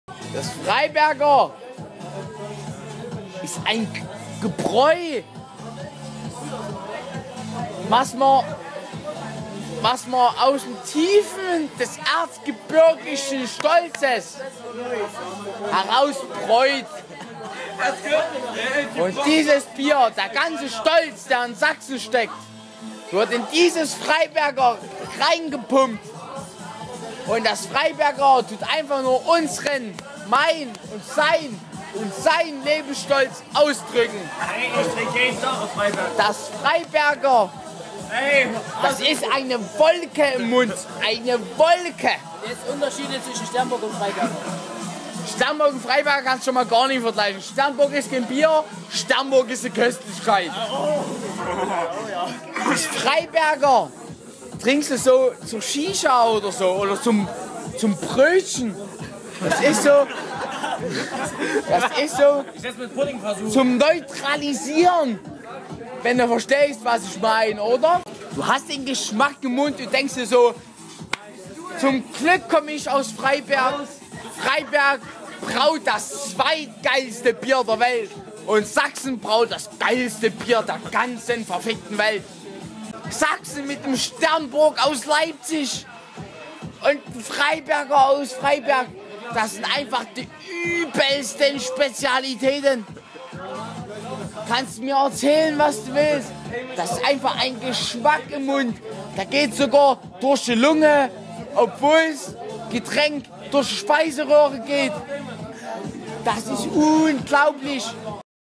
Bierkunde mit Mr. Sukuta | Freiberger und Sternburg-Bier | mandarinemangoTV
Heute erklärt euch Mr. Sukuta mal was über Bier. Viel Spaß!